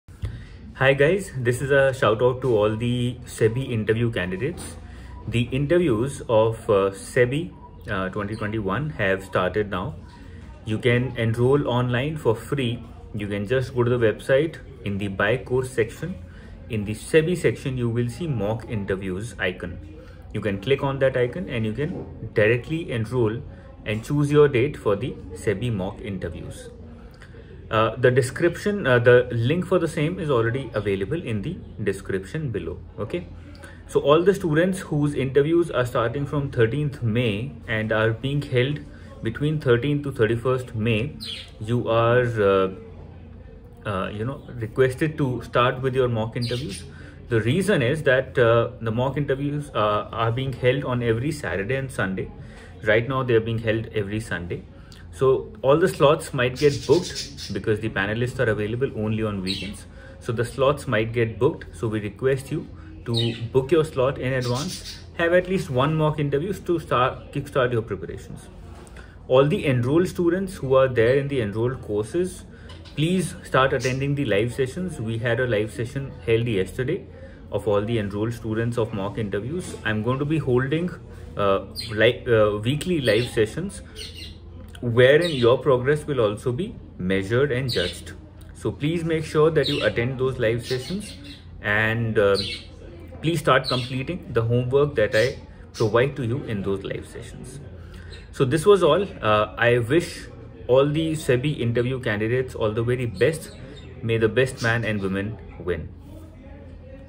SEBI Grade A Interview Guidance | Book FREE SEBI Mock Interview Slot
SEBI Grade A Interview Admit card is out and now is the time to divert all the attention towards the mock interviews of the concerned examination.
The time is less and in the remaining period, I want all the aspirants who have qualified for the interview round to give as many mock interviews as possible. To facilitate this, I have established a provision where the qualified candidates can register themselves for FREE and schedule their mock interviews at their convenience.
The mock interviews will be provided free of cost and will be conducted by a team of industry experts. Moreover, following the culmination of mock interviews, the candidates will be given a thorough analysis of their performance and tips to improve.
Register for the mock interviews now. Here’s the link to that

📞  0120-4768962